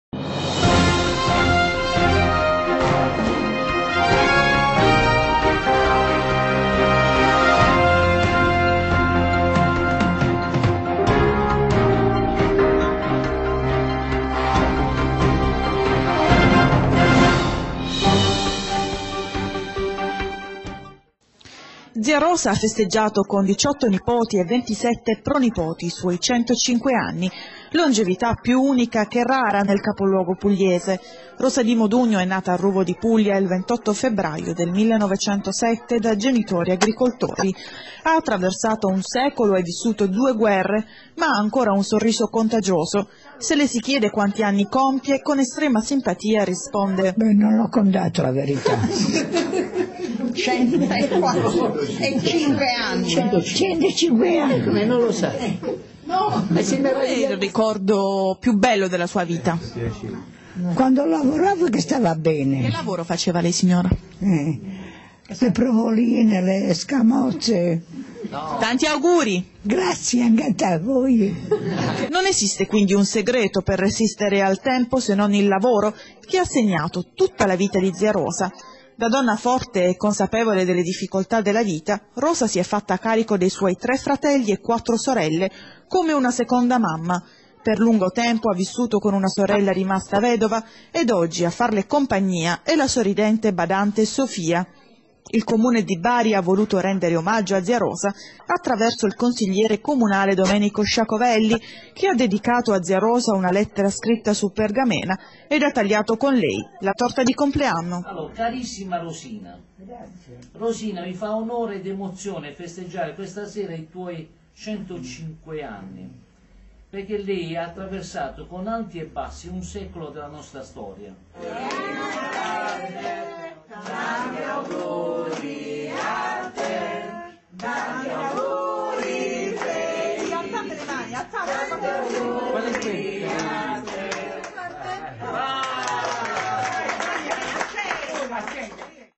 ZIA ROSA HA COMPIUTO 105 ANNI MA HA ANCORA TANTA VOGLIA DI SORRIDERE ALLA VITA
Zia Rosa ha festeggiato con 18 nipoti e 27 pronipoti i suoi 105 anni. Longevità più unica che rara nel capoluogo pugliese. Rosa Di Modugno è nata a Ruvo di Puglia il 28 febbraio del 1907 da genitori agricoltori. Ha attraversato un secolo e vissuto due guerre ma ha ancora un sorriso contagioso. Se le si chiede quanti anni compie, con estrema simpatia risponde che non li ha contati. Nella vita ha lavorato in un'azienda casearia e le maggiori soddisfazioni le ha avute quando stava bene e poteva camminare. Non esiste quindi un segreto per resistere al tempo se non il lavoro, che ha segnato tutta la vita di zia Rosa. Da donna forte e consapevole delle difficoltà della vita, Rosa si è fatta carico dei suoi tre fratelli e quattro sorelle come una seconda mamma. Per lungo tempo ha vissuto con una sorella rimasta vedova ed oggi a farle compagnia è la sorridente badante Sofia. Il comune di Bari ha voluto rendere omaggio a zia Rosa attraverso il consigliere comunale Domenico Sciacovelli, che ha dedicato a zia Rosa una lettera scritta su una pergamena ed ha tagliato con lei la torta.